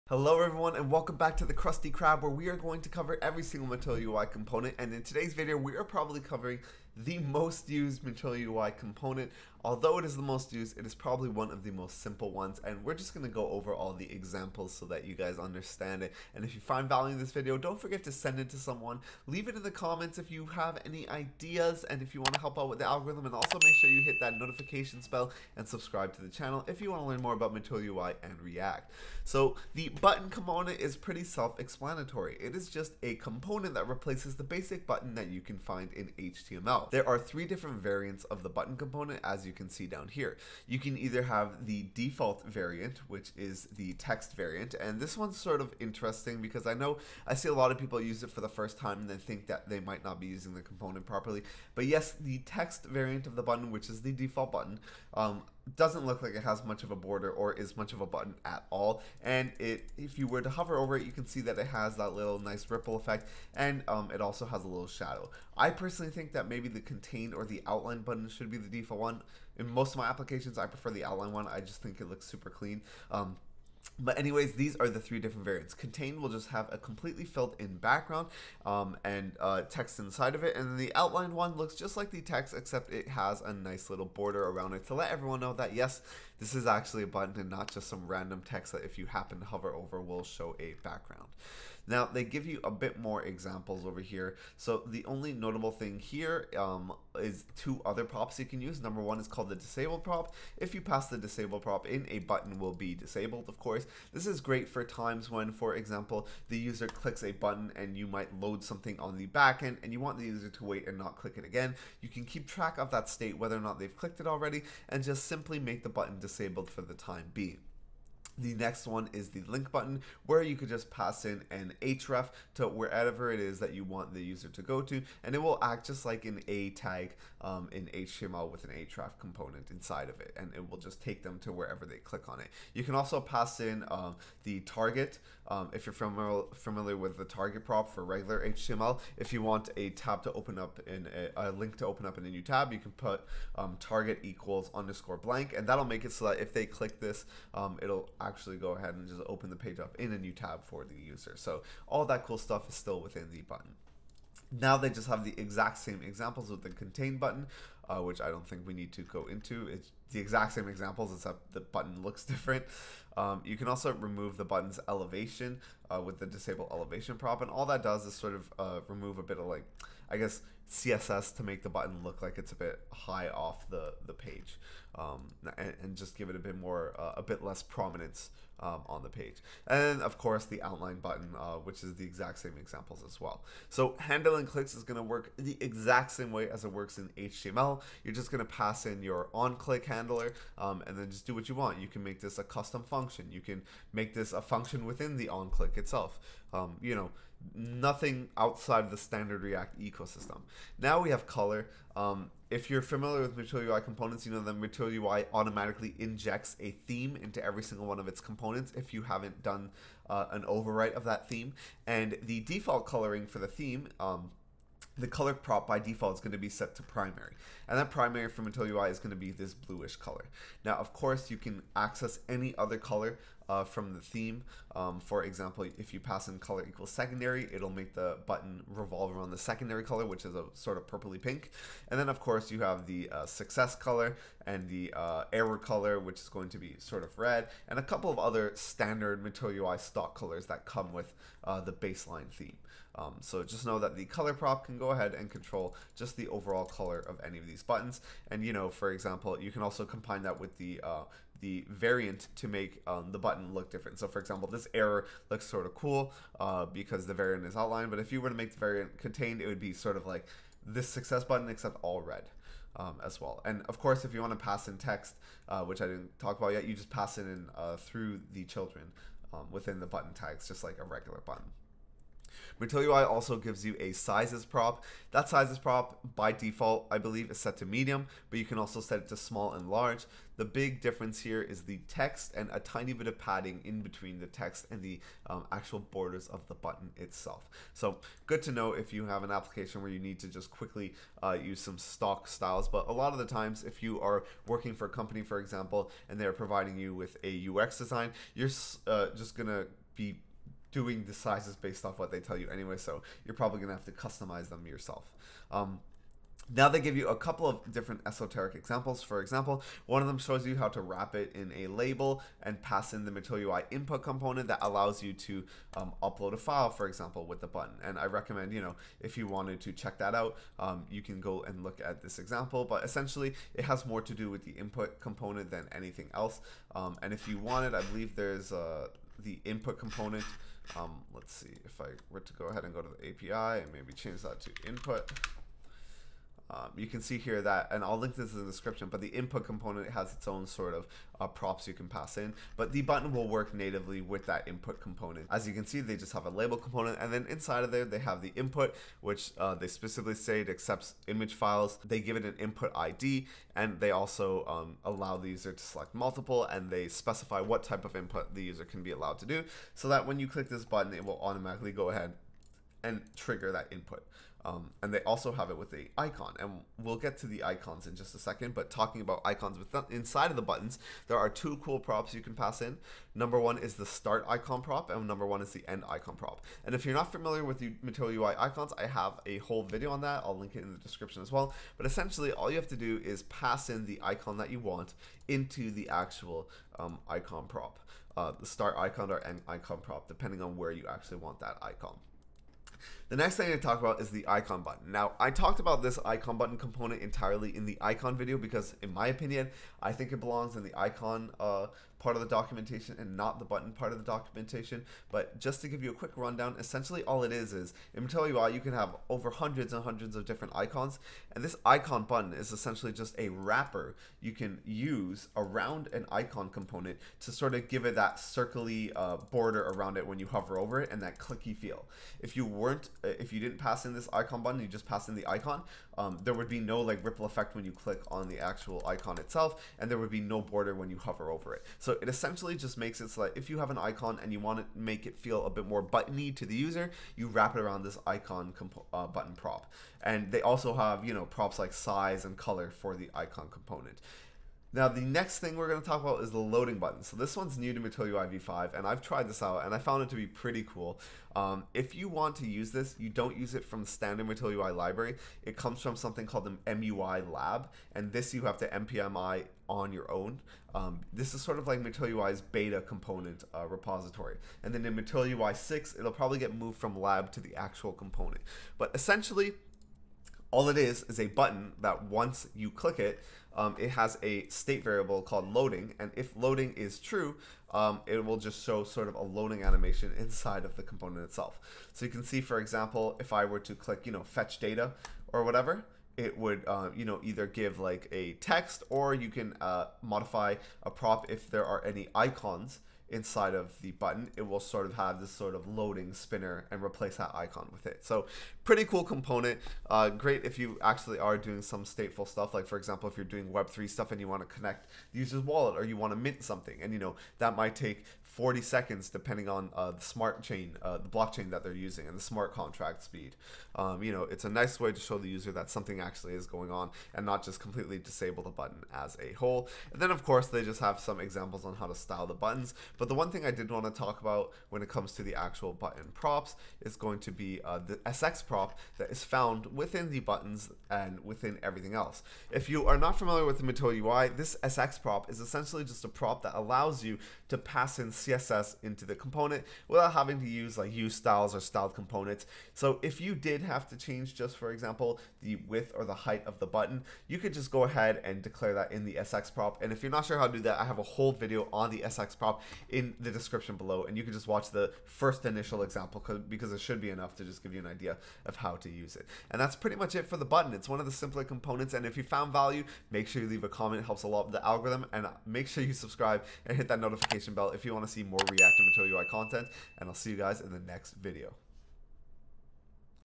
MUI V5: Button (and the new Loading Button)
In this video we go over one of the most used, and simplest Material UI V5 components there is, the Button component! This Button is a great replacement for React / HTML's standard Button, and offers you a wide variety of use cases and props. We also talk a bit about @MUI/Lab's LoadingButton that's new to MUI V5.

-TIMESTAMPS-
0:00 Intro
0:40 Variants (Text, Outlined, Contained) and Disabled / Href / Target Props
3:00 Onclick handlers
3:40 Color prop (theme)
4:31 Size props (small, medium, large)
5:06 Upload button (button with Input)
6:05 Adding icons inside your button
6:35 IconButton component (wrapping icons with button effects)
7:25 The LoadingButton component (great for lots of React usecases)
8:45 Styling buttons with the SX Prop